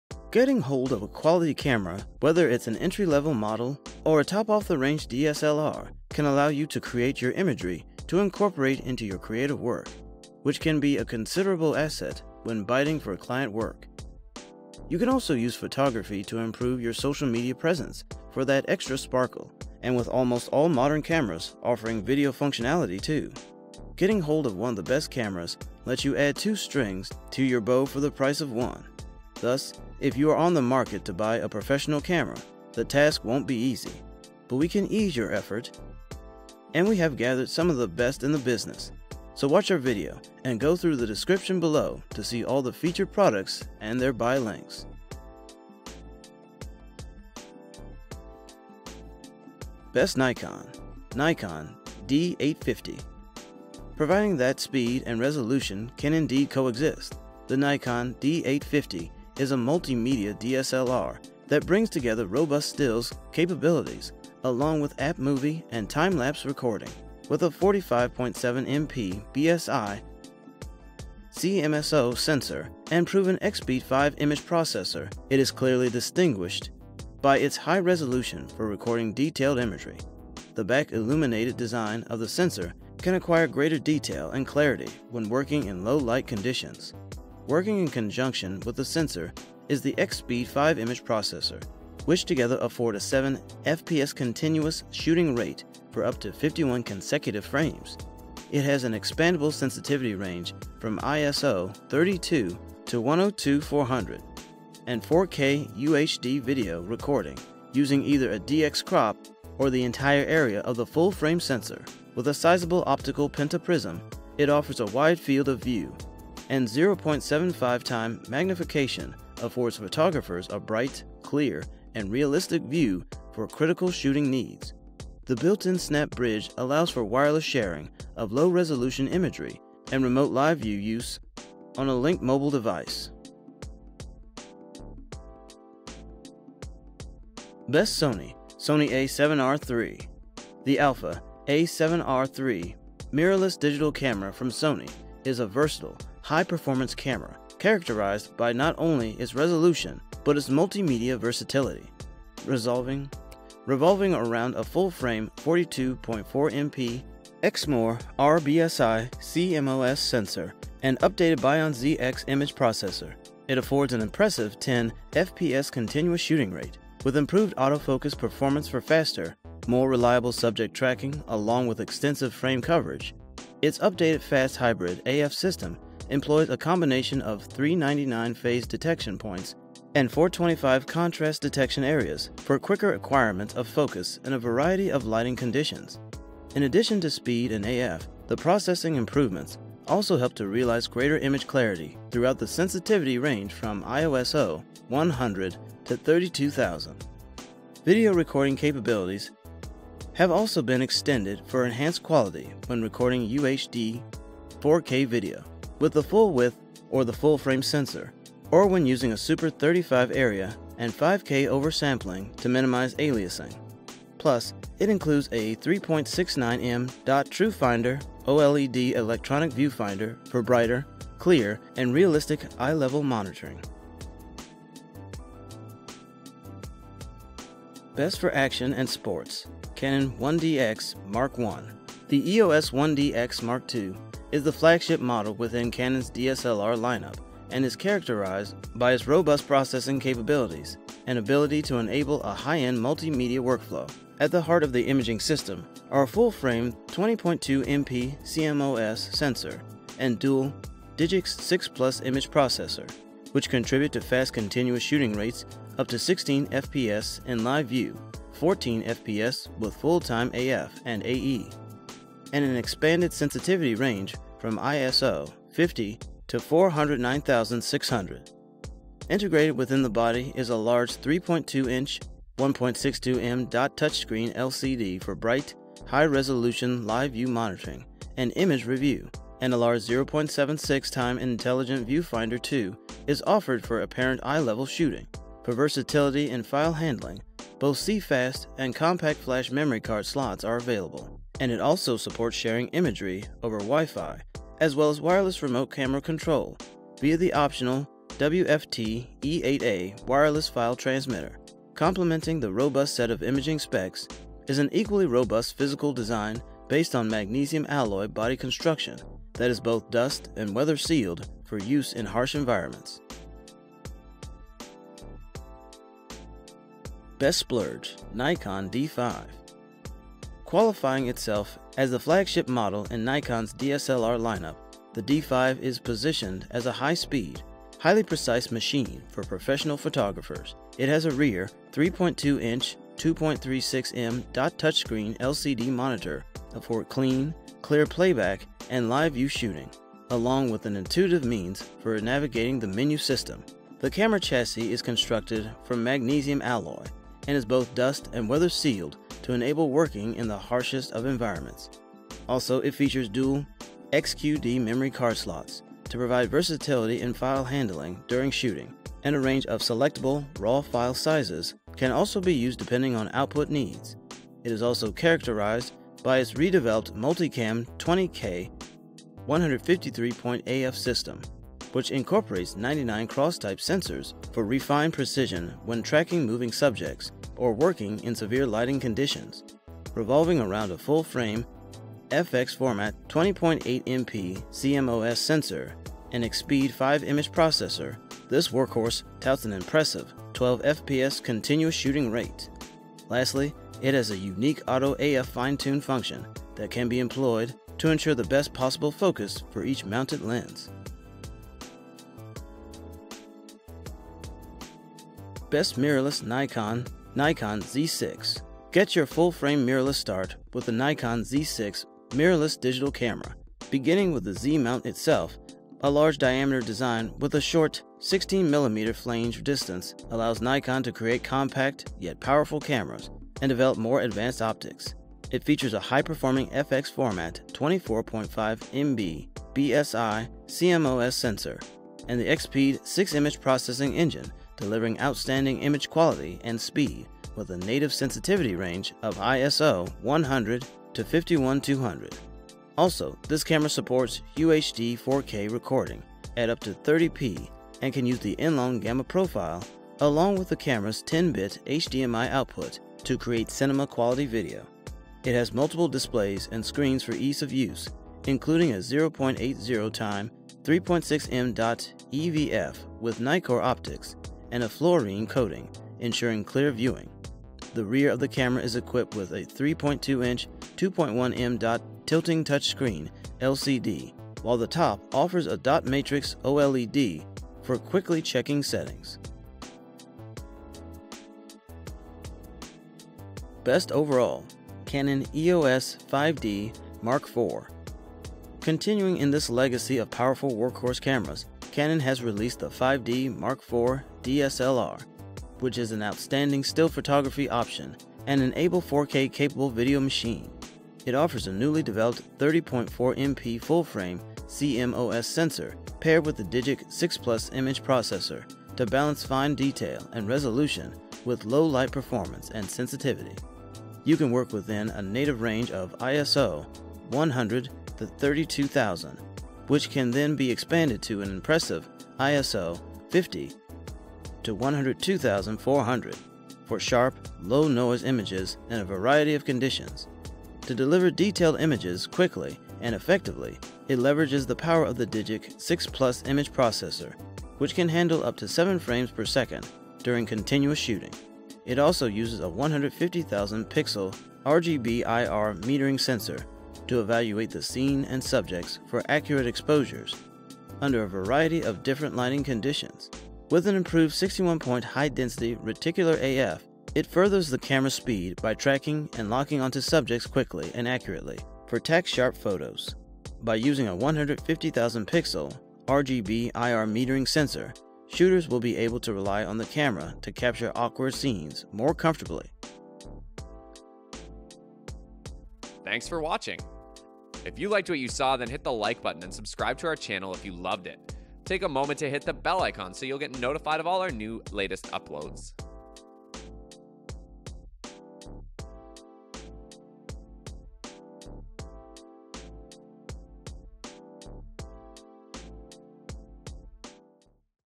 BEST PROFESSIONAL CAMERAS!
1. Canon E0S 5D Mark IV
2. Nikon Z6
3. Nikon D5
4. Canon 1DX Mark I
5. Sony a7R 111
6. Nikon D850